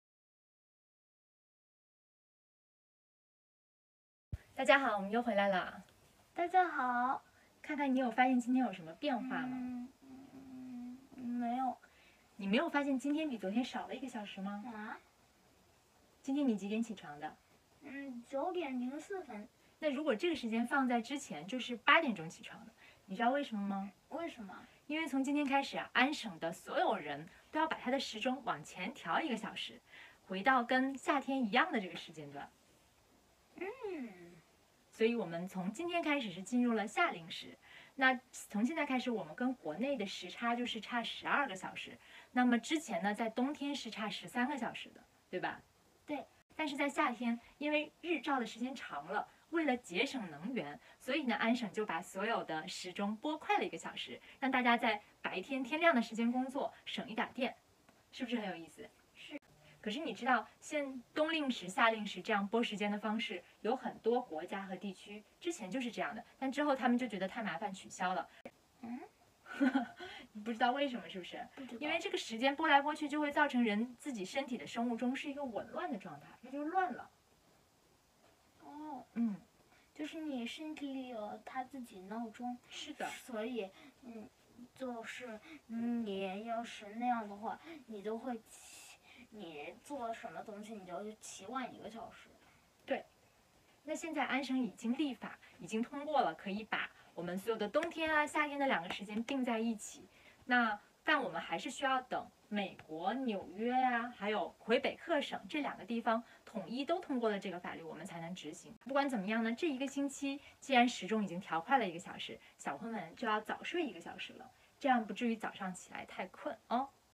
凭空消失的一小时去哪儿了？加拿大这个省最后一次调时钟?
今天，安省所有人的表都被调快了一个小时，我们正式进入到了夏令时，现在的时间跟国内刚好相差12小时，原来的夏令时是为了节省能源，让人们在日光充足的时候多工作，但现在，每年两次调整时间带来了更多的麻烦，也引起了很多人的身体健康问题。安省已经通过了立法，只等着邻居：美国纽约州和魁北克省通过，就可以永久停留在夏令时了，这会是我们最后一次调时钟吗？